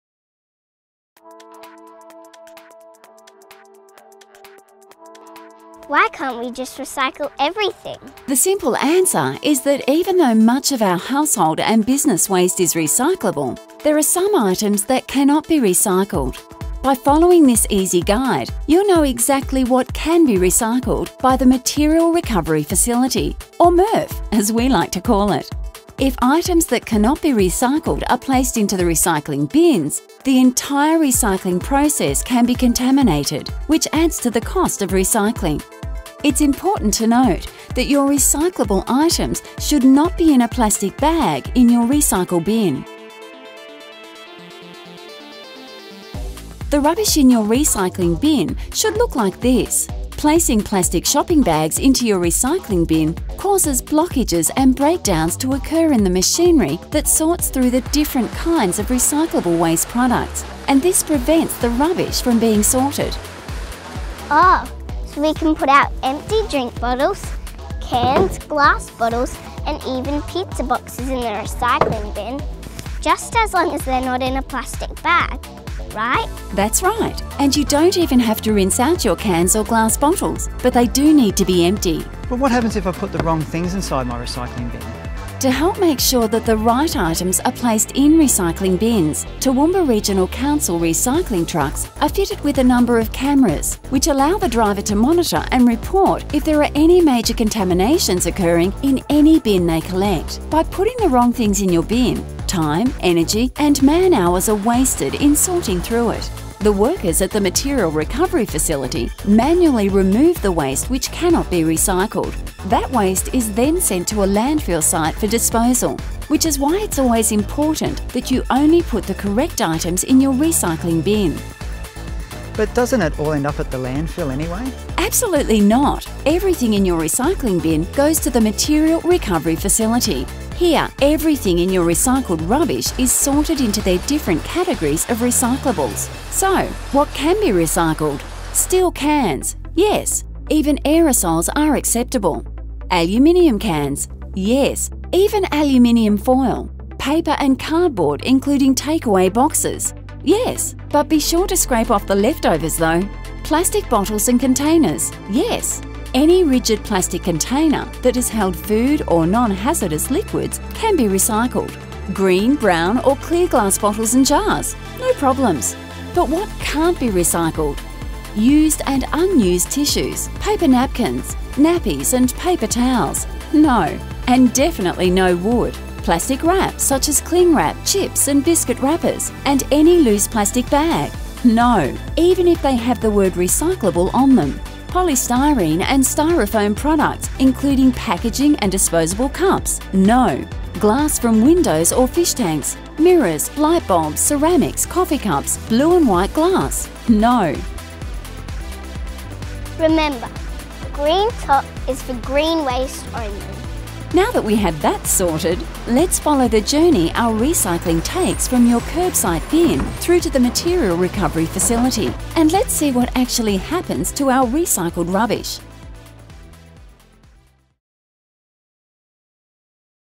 What can you recycle?
Welcome to our series about waste and how it is dealt with in the Toowoomba Region. This video focuses on what can and can't be recycled.